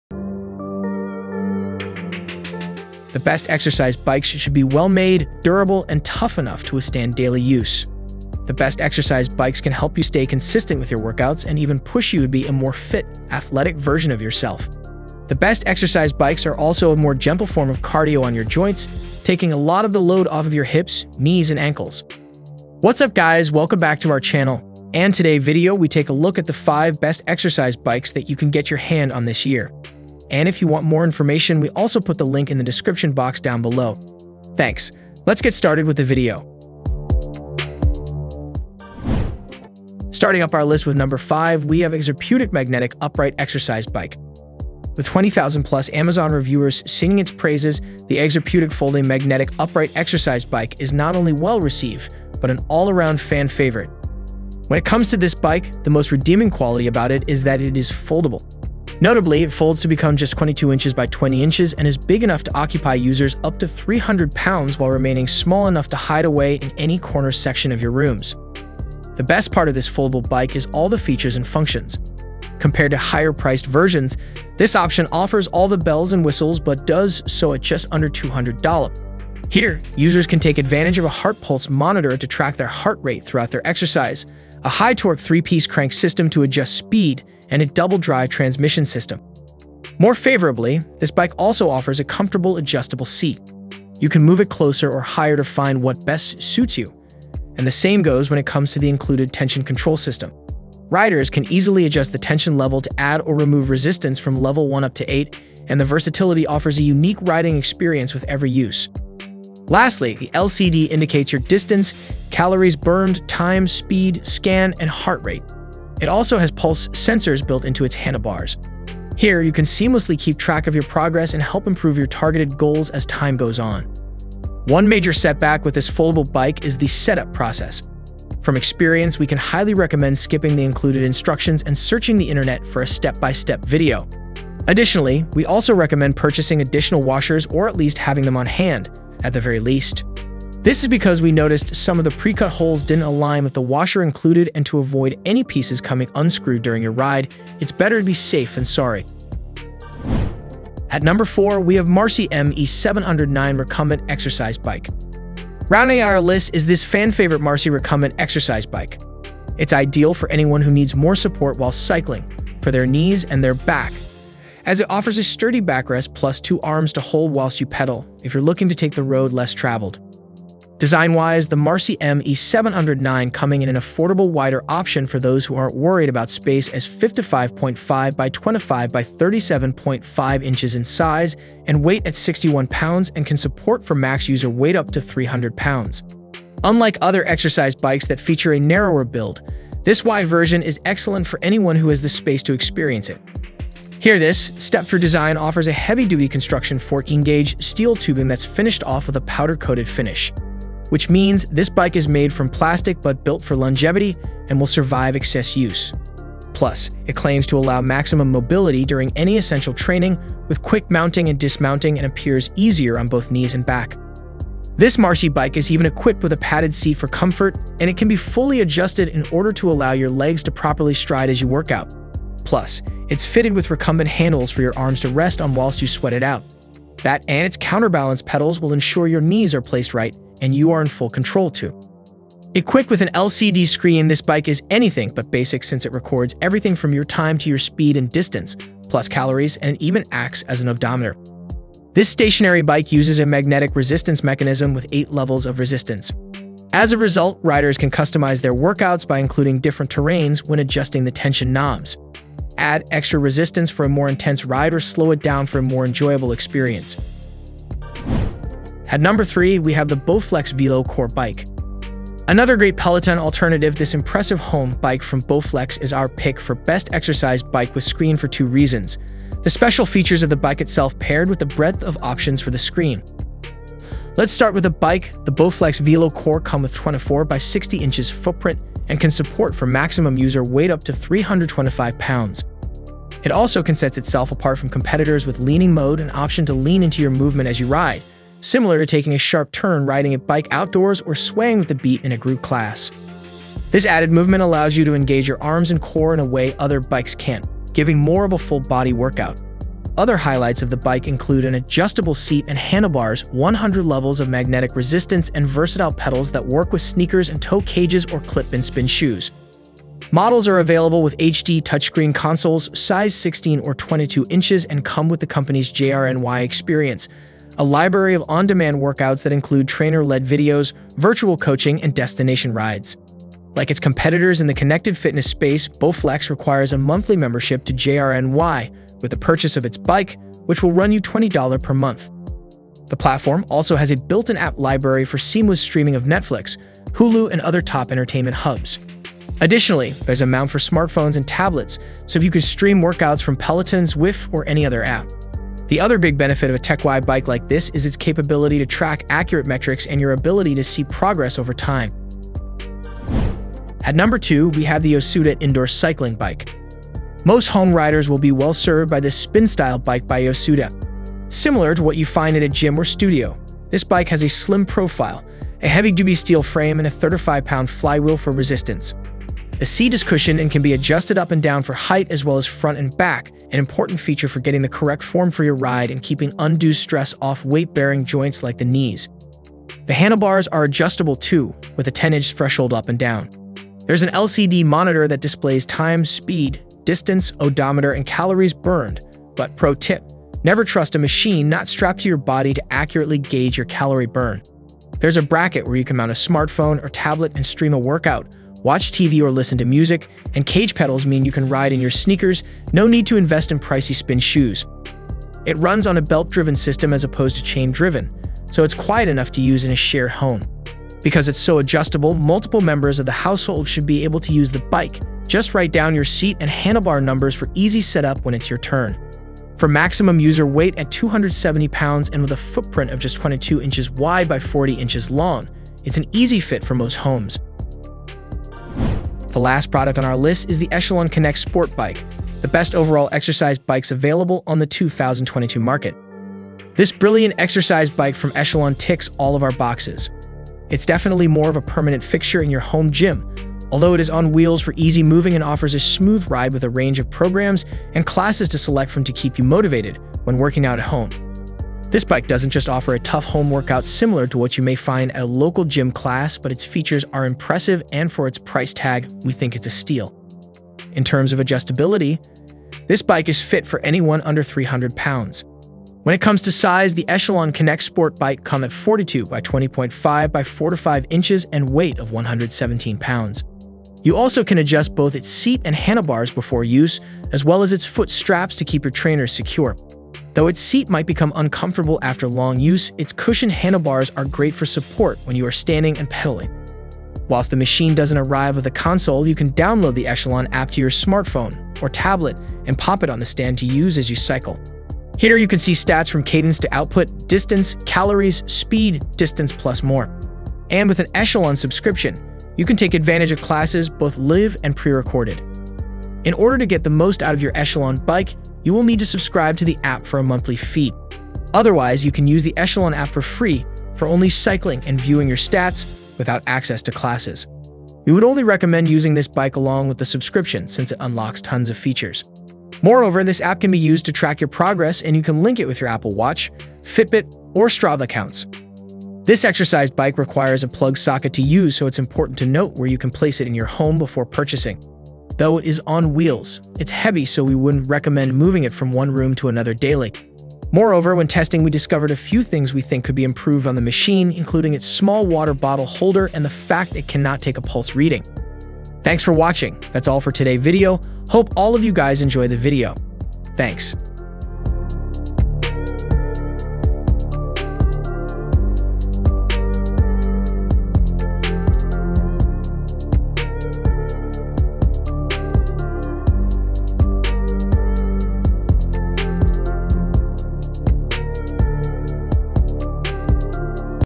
Best Indoor Bike of 2022 | The 5 Best Exercise Bikes Review
▶ Here Are The Link Of The 5 Best Exercise Bikes of 2022 That you can buy on Amazon

▶5- Exerpeutic Magnetic Upright Exercise Bike

▶4- Marcy ME-709 Recumbent Exercise Bike

▶2- Yosuda Indoor Cycling Bike

▶1- Echelon Connect Sport Bike

The best exercise bikes should be well-made, durable, and tough enough to withstand daily use. The best exercise bikes can help you stay consistent with your workouts and even push you to be a more fit, athletic version of yourself. The best exercise bikes are also a more gentle form of cardio on your joints, taking a lot of the load off of your hips, knees and ankles.